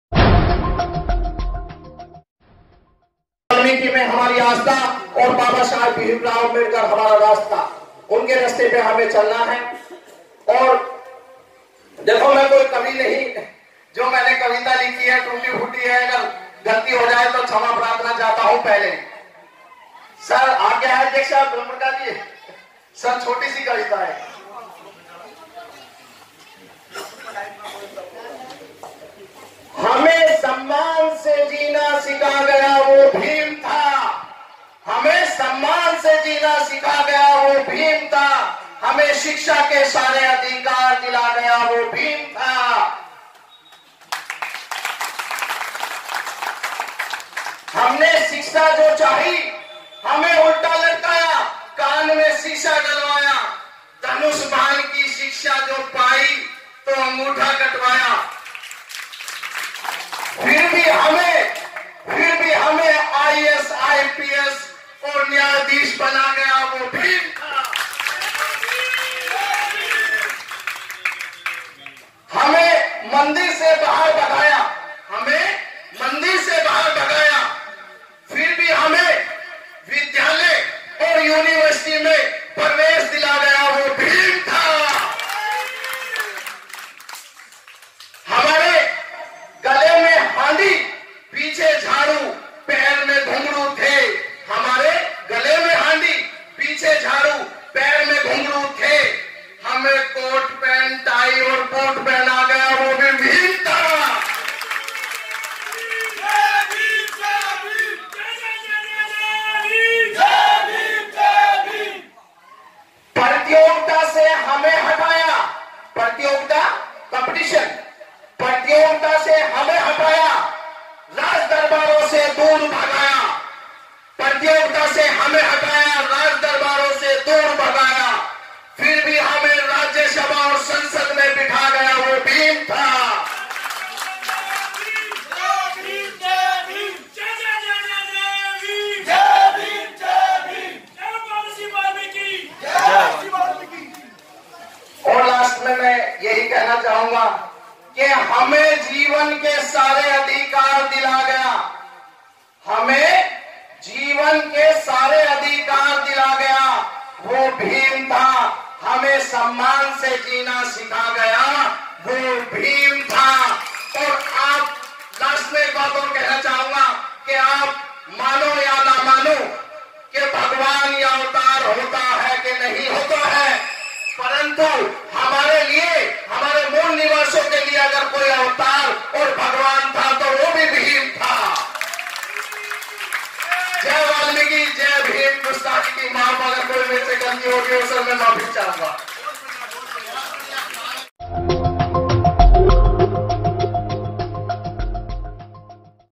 हमें IS PCS बना गया वो भीम था
देखिए यह कविता सुनते ही पूरा का पूरा बाल्मिकी समाज कैसे  जाग गया।

Story by- Samtaawaz Tv
Editor- Ranveer Antani
Reporter- Manoj antani
Cameramen-Manoj Antani
हमें सहायता करने के लिए हमारे बैंक की डिटेल
Manoj kumar: Ac.No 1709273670 Ac.No.
Sentral bank of india
 Ifsc cbin 0284023